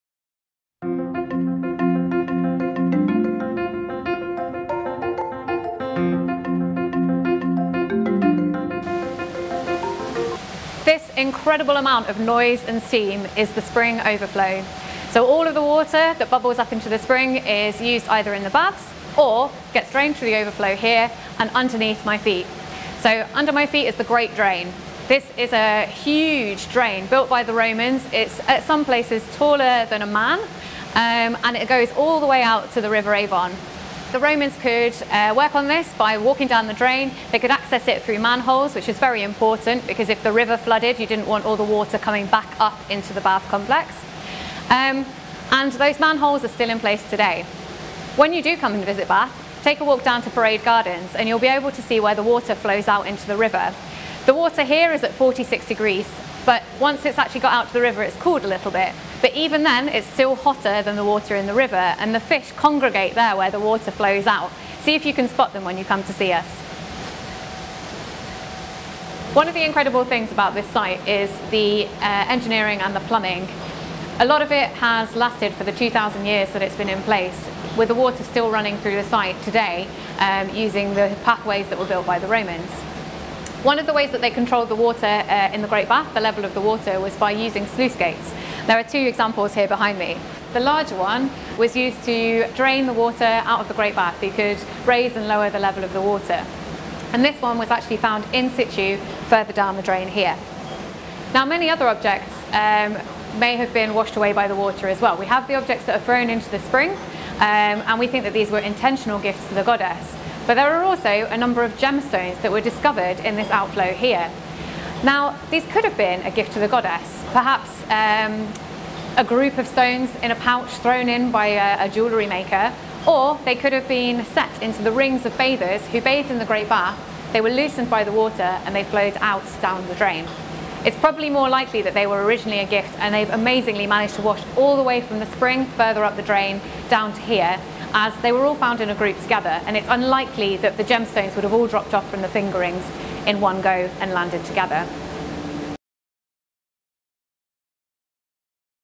Discover a 2000-year-old drain at The Roman Baths
Learn about the amazing Roman engineering and plumbing which is still in use at the Roman Baths 2000 years later! Discover some real hidden gems that were uncovered at the site 💎